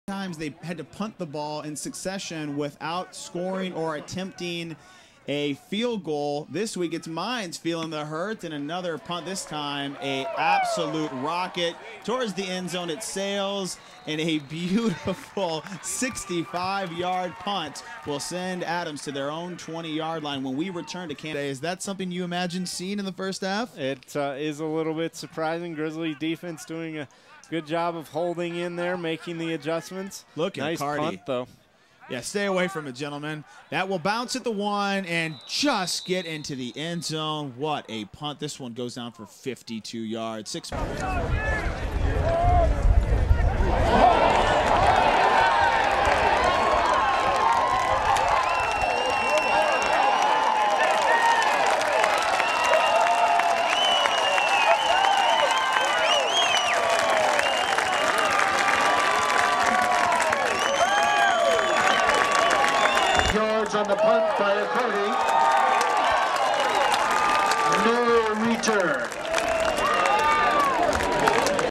Taylor Accardi, Colorado Mines P
Highlights of Taylor Accardi, Colorado Mines punter (#16) who will be at RMAC Football Media Day, August 1, at the Colorado Springs Marriott Hotel.